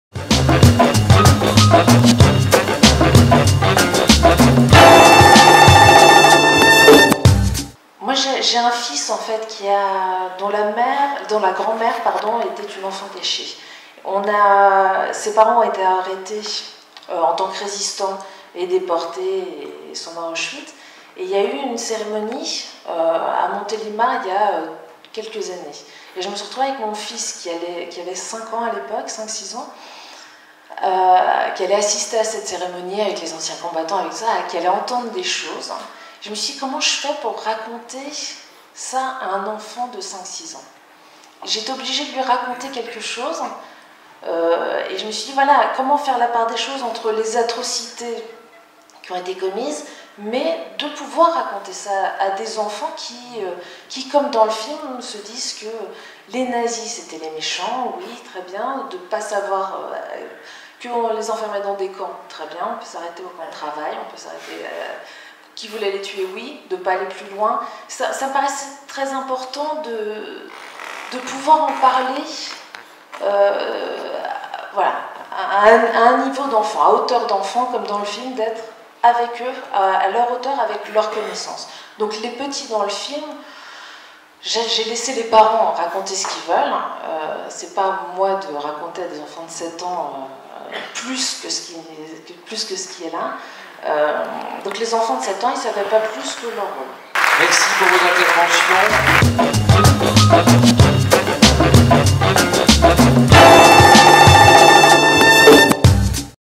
Lola DOILLON - Rencontres du Cinéma de Gérardmer 2016
pour son film "le Voyage de Fanny"